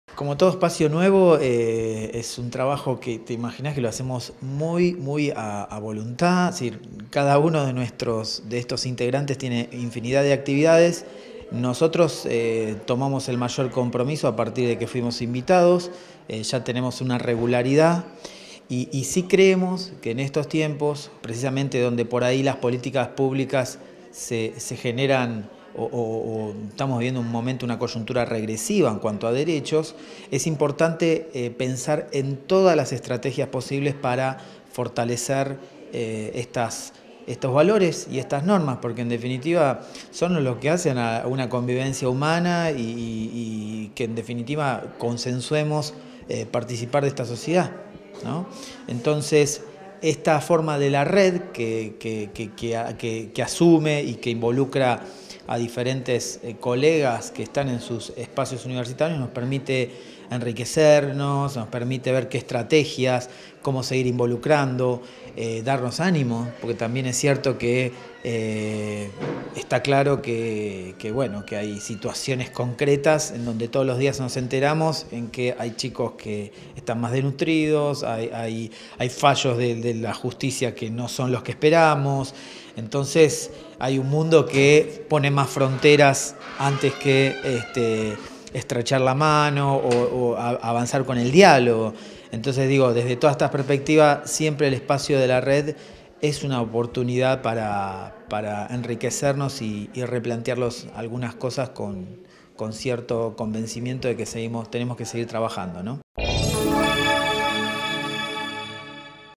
REUNION INTERUNIVERSITARIA DD. HH. - MATIAS PENHOS
DESTACAN LA ARTICULACIÓN PARA EL DESARROLLO DE ACCIONES.

Facebook.com/uniriotv
Twitter.com/uniriotv
Instagram.com/uniriotv
Web - uniriotv.unrc.edu.ar
o envianos un email a
UniRio TV
Universidad Nacional de Río Cuarto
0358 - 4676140